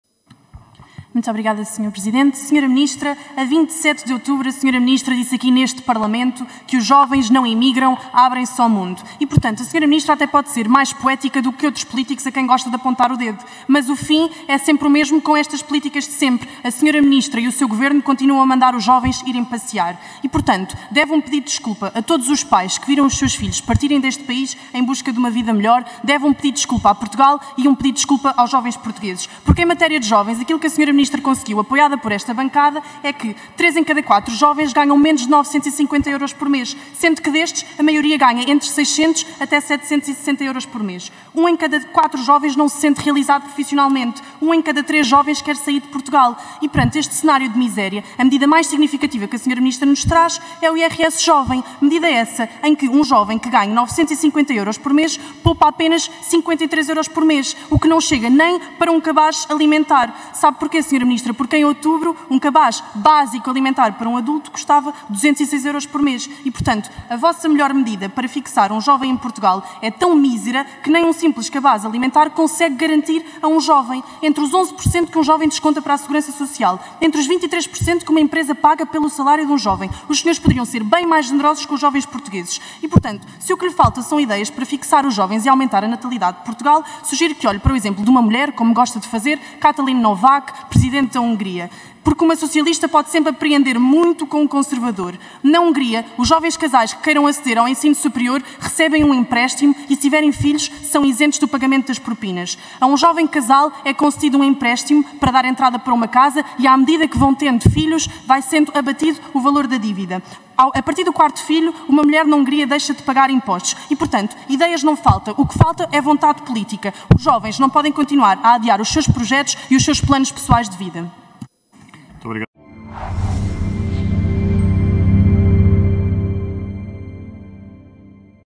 💃🏻🇵🇹A Deputada Rita Matias arrasa uma ministra que “manda os jovens passear”!
A Deputada Rita Matias arrasa uma ministra que “manda os jovens passear”! Um em cada três jovens querem emigrar pois os salários, mais os impostos, são ridículos para os jovens deste país! A deputada do Chega ainda deixa conselhos e exemplos da Presidente Katalin Novák da Hungria 🇭🇺, para fixar jovens e fazer disparar a natalidade como aconteceu naquele país conservador!